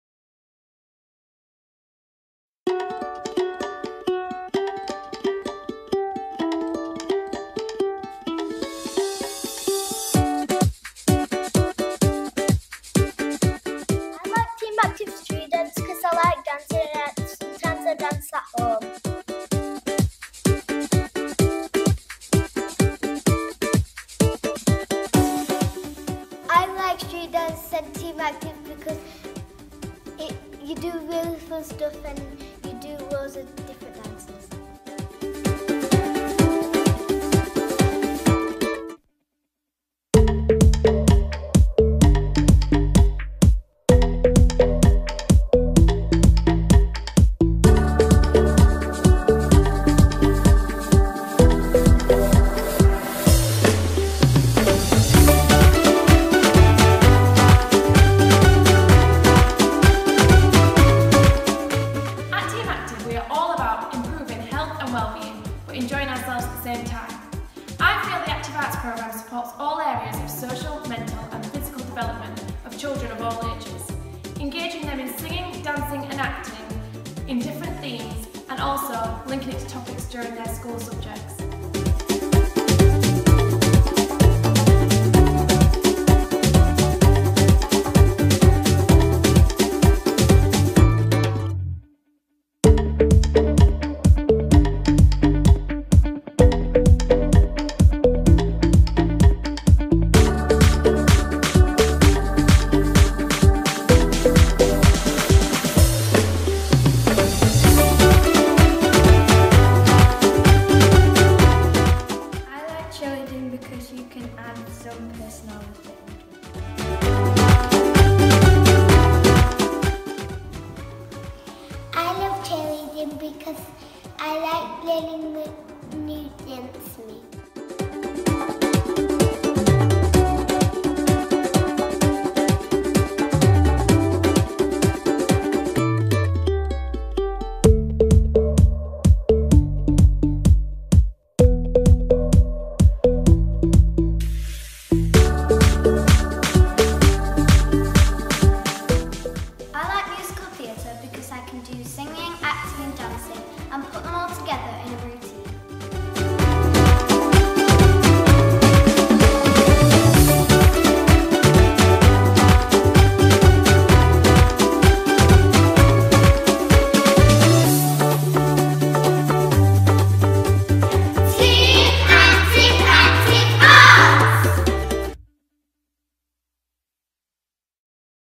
Activ Arts | Sing, Dance & Be ACTive
Engage children in a unique dimension of PE & the Arts to improve resilience, social development and confidence. Providing & promoting exploration & self-expression through a range of media including movement, music, art & cultural diversity.